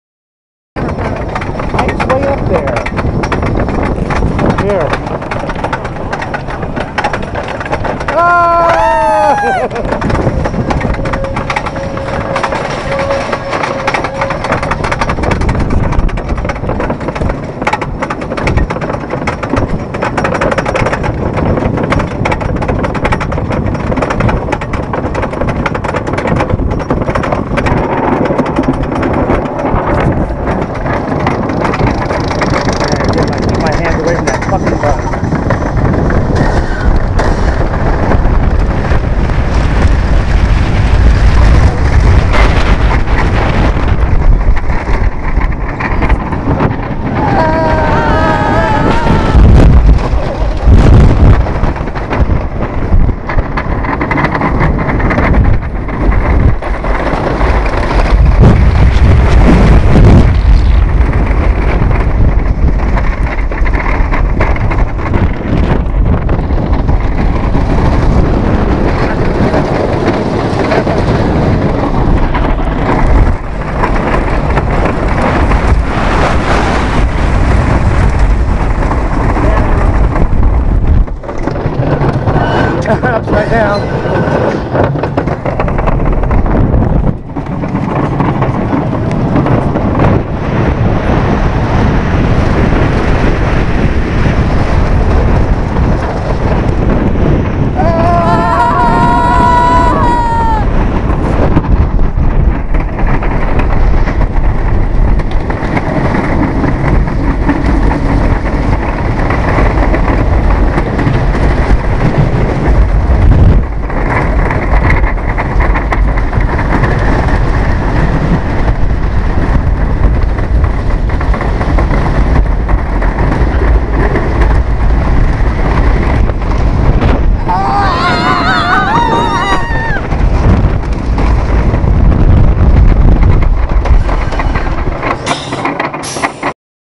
Mean Streak Rollercoaster Front Seat Couple Go Crazy
Me And April On Mean Streak Rollercoaster Front Seat At Cedar Point In Sandusky, Ohio On June 23, 2010